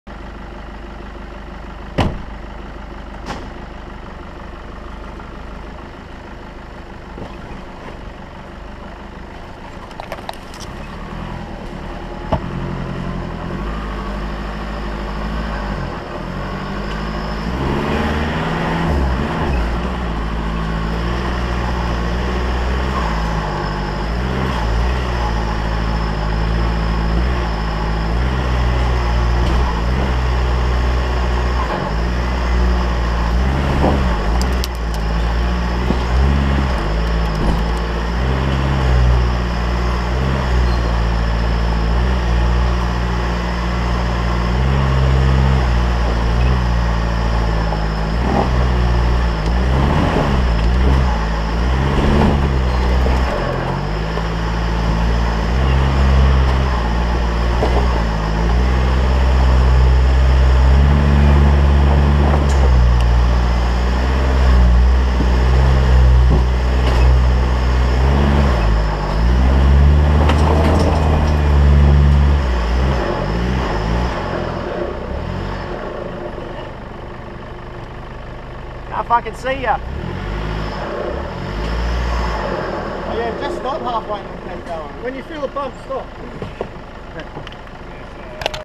Staples line Brunswick 2014 4wding
Staples line 23/5/14. Nice little section, pretty slippery.